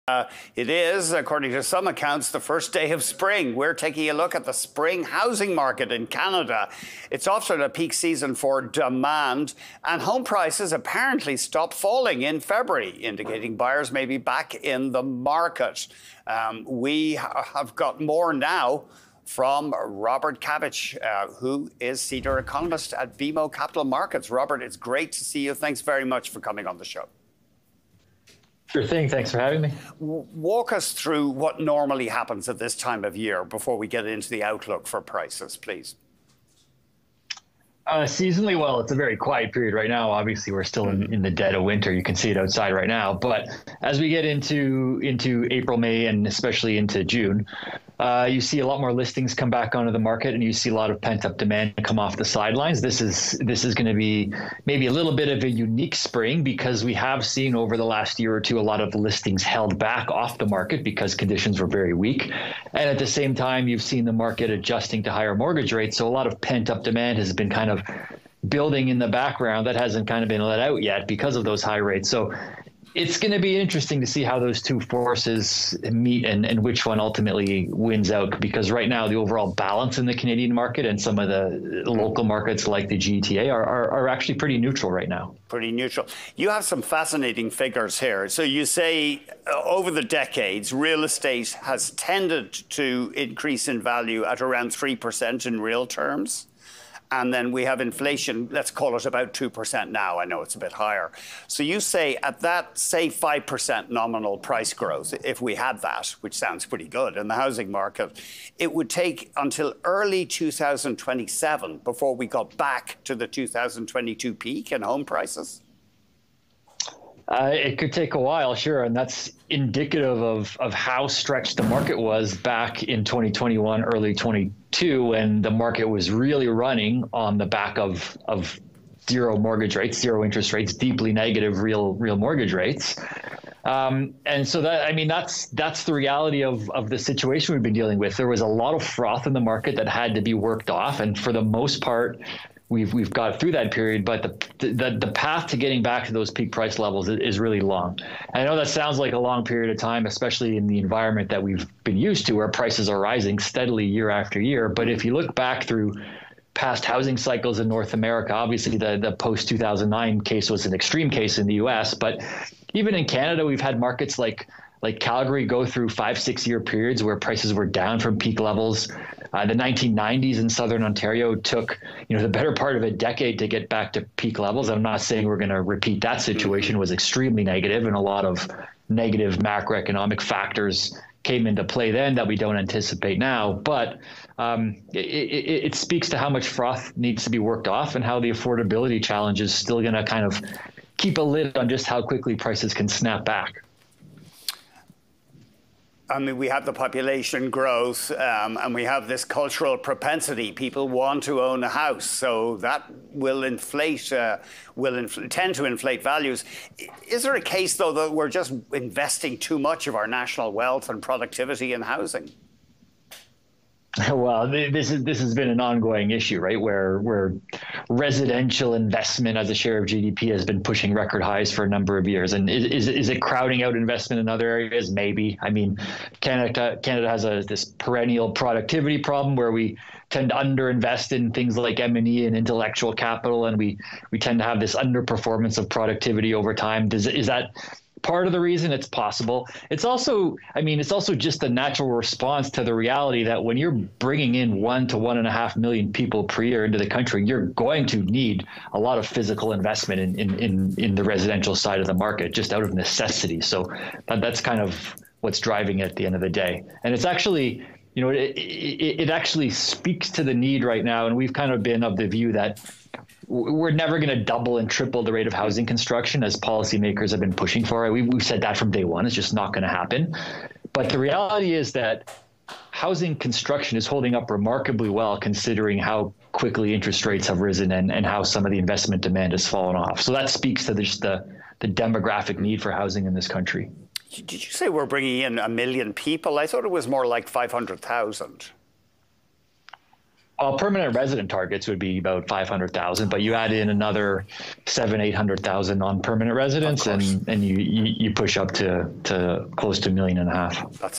It will be a while before we see peak 2022 Canadian home prices again: Robert Kavcic
Robert Kavcic, senior economist at BMO Capital Markets, joins BNN Bloomberg to discuss the spring housing market in Canada and his outlook for demand as buyers come off the sidelines.